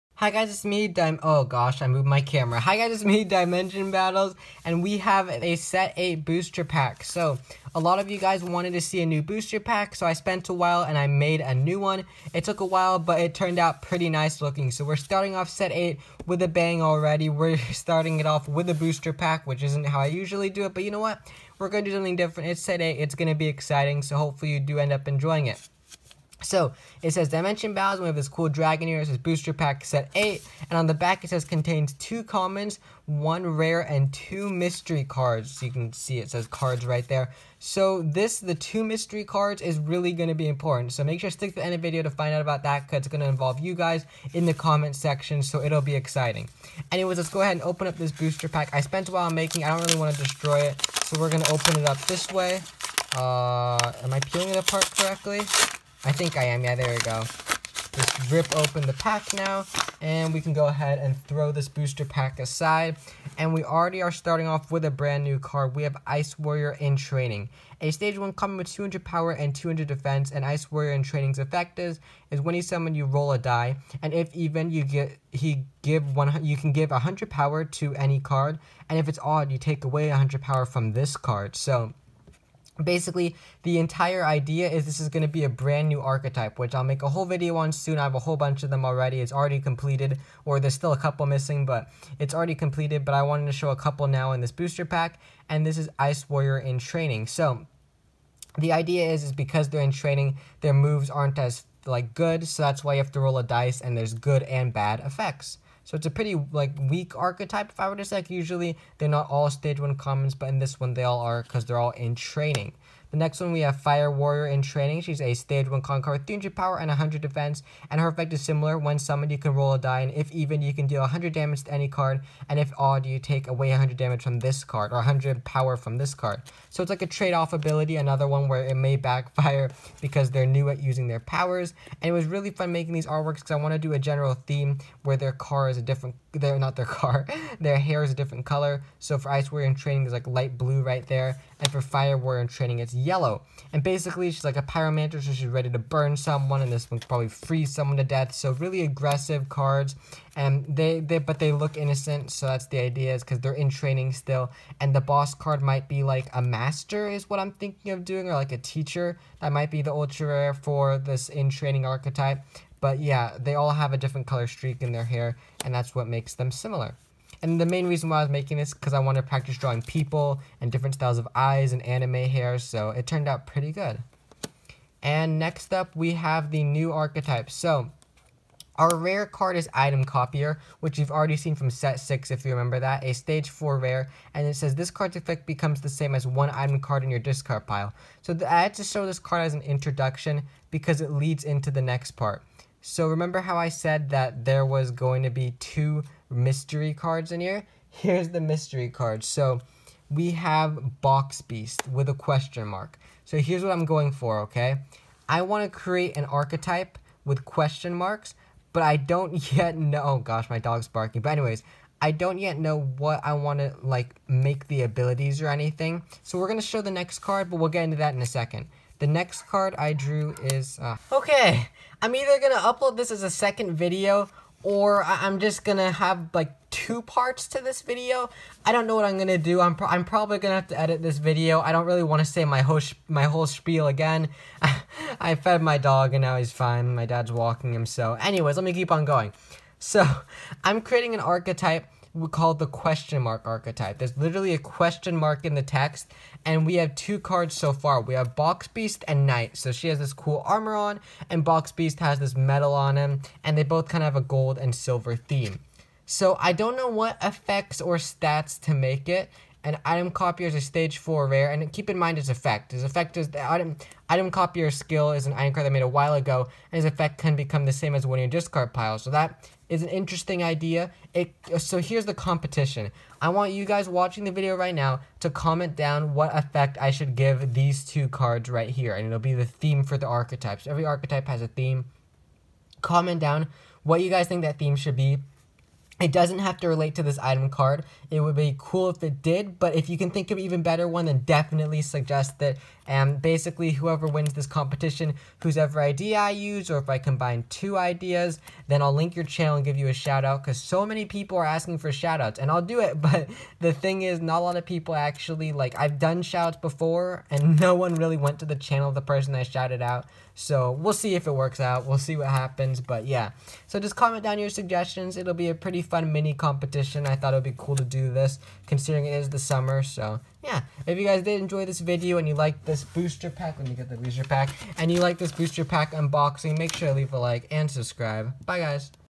Homemade tcg Booster Pack Unboxing
In this Dimension Battles video I unbox my homemade trading card game booster pack.

Comment down bellow your suggestions for the "?" archetype.